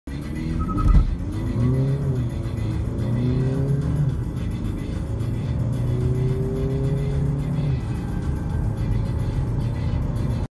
WV PASSAT 1.6 TDİ OTOMATİK 120 HP 0-100 HIZLANMA
wv passat  ile 0 dan 100 kaç saniyede ulaştık?